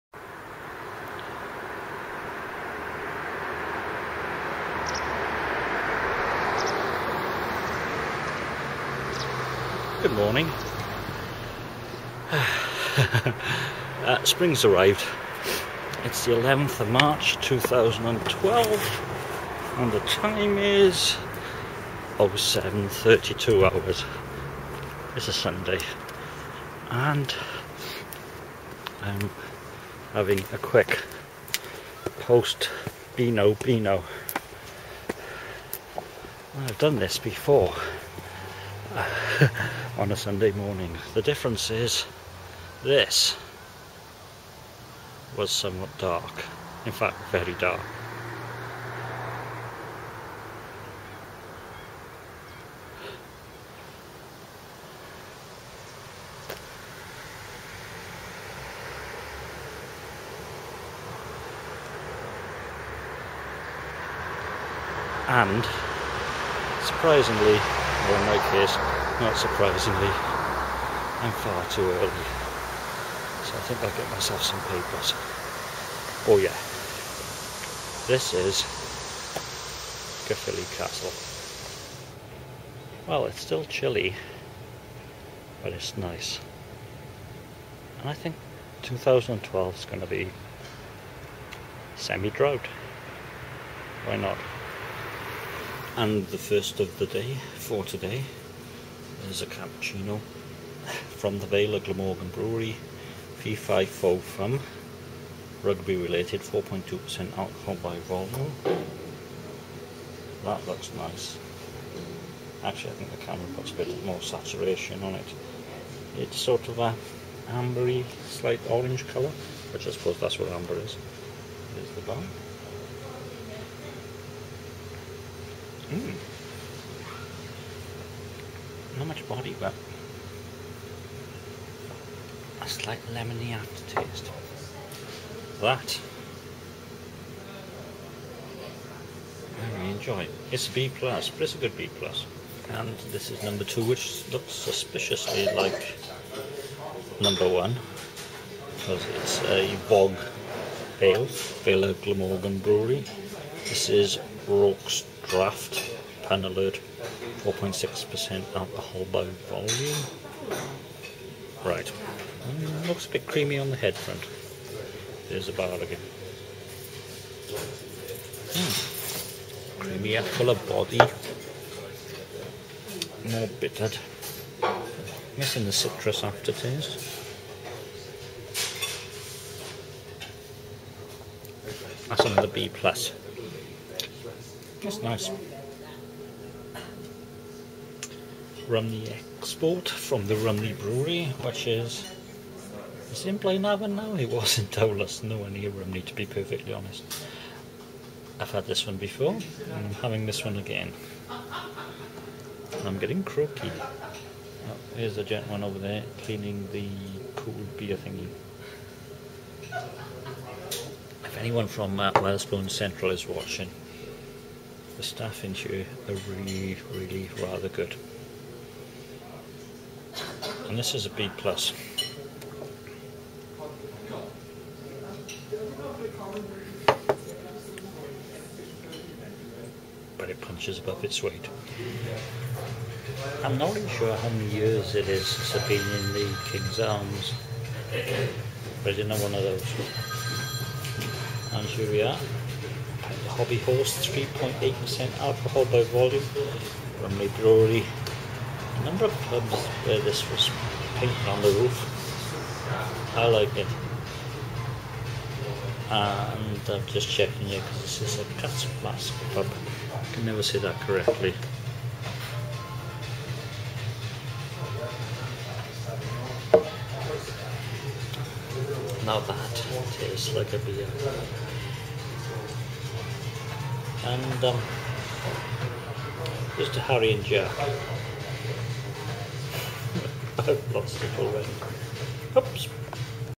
Spring
Post birthday but still in the need for a beano.  Bright but still chilly.  The Castle looks good.  All Welsh ales and two pubs towards my ale trail fridge magnet.